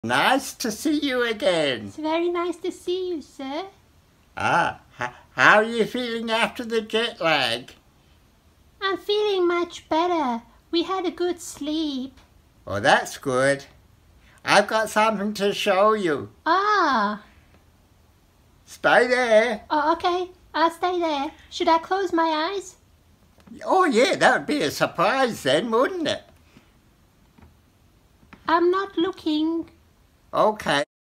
Crazy Bear's House 🐻🏠😃
Koala Steve and the llama have now returned to Australia and they're visiting the crazy bear they've met on their travels.